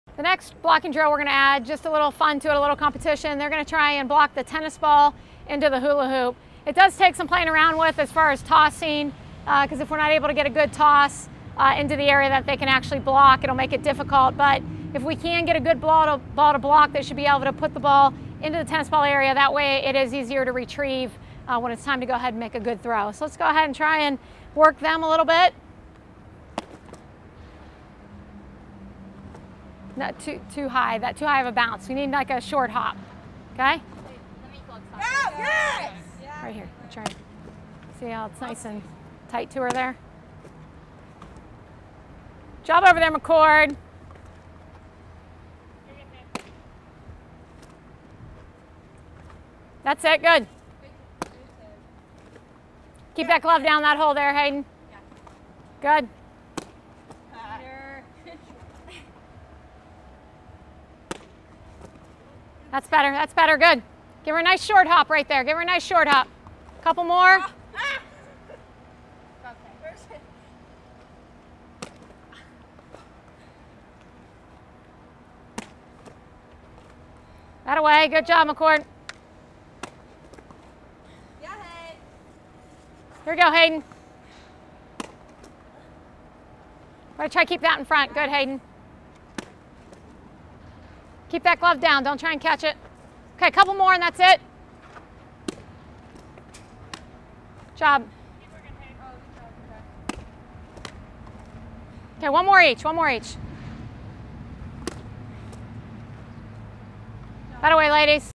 35 Competitive Drills to Build a Complete Catcher
with Shonda Stanton, Marshall University Head Coach; the winningest coach in Marshall softball history; 2013 Conference USA Tournament Champions; 3x Conference champions; former Coach for the Akron Racers of the Women's Professional Fastpitch League. Catchers have a wide variety of skills they must master to be successful, including receiving, blocking, and throwing out runners. In this video, Coach Stanton gives you drills to mold your athletes into complete catchers.